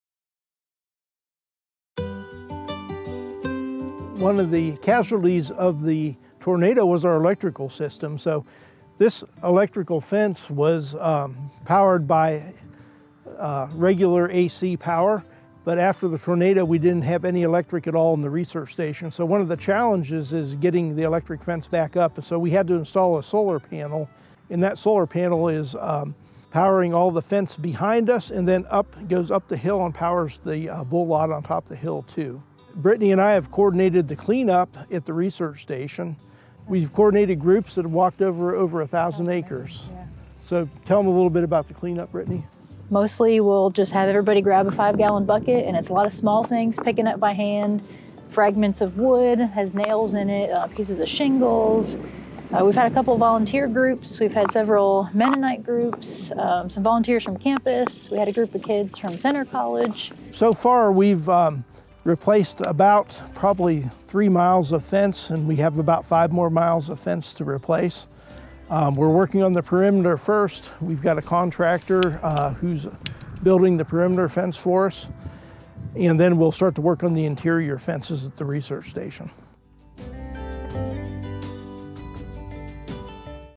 Princeton Tornado Damaged Fence Repair
Dr. Chris Teutsch and Brittany Hendrix have been busy coordinating the tornado clean up at Princeton Research & Education Center while also setting up a solar panel to charge the electric fencing on the station and mending damaged fences.